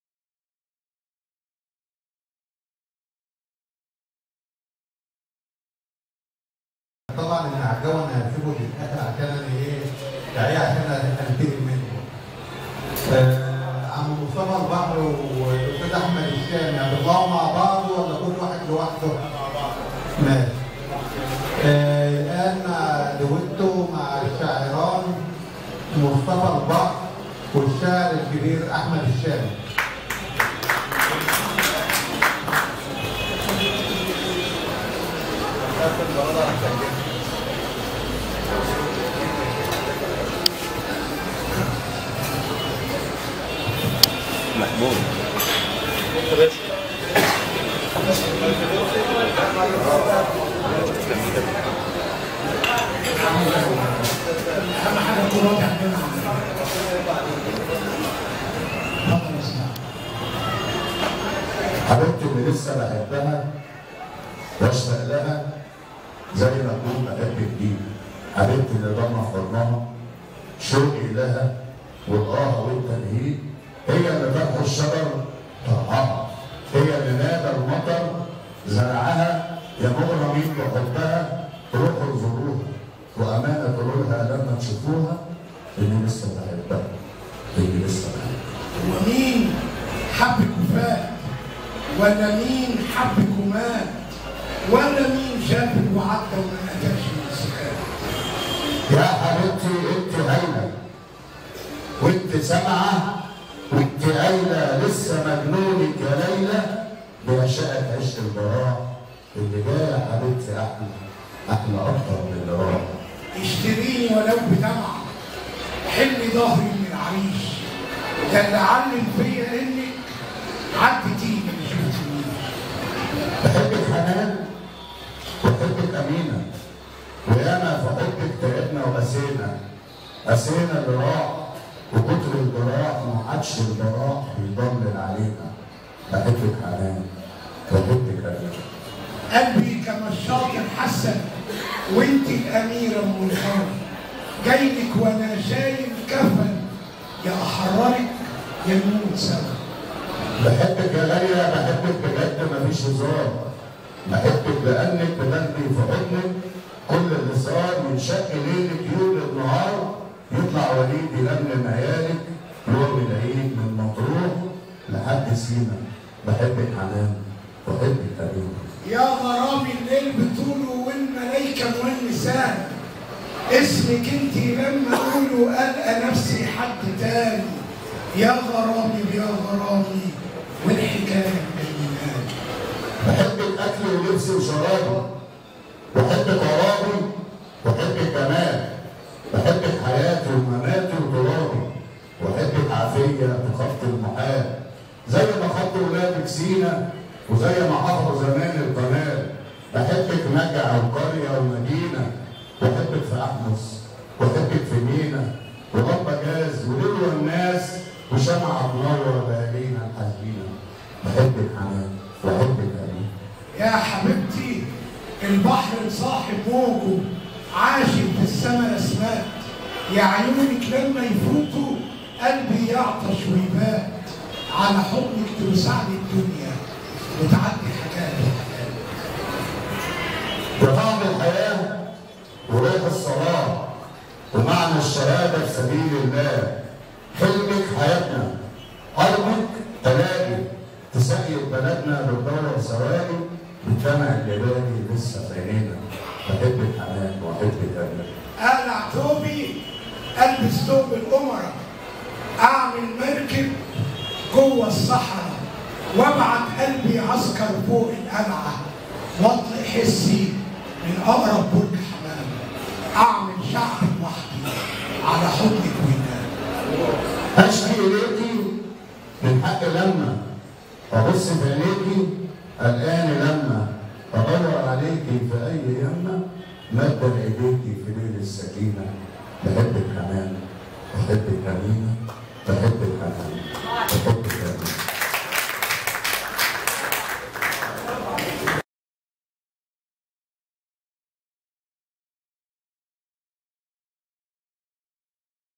ندوة صالون صوت مصر/الشاعران/مصطفى البحر وأحمد الشامي/الجمعة26/11/2021/هيئة خريجي الجامعات بالقاهرة
أمسيةٌ ثقافيةٌ
ندوة صالون صوت مصر
مؤسس ورئيس مجلس إدارة الصالون
الشاعر / رضا أبو شنب
مشاركة الشاعر / مصطفى البحر
والشاعر / أحمد الشامي
المكان : جمعية هيئة خريجي الجامعات
شارع محمد بك الألفي رقم 11/ عرابي
أسوان / عنيبة الخير / القاهرة
 مصر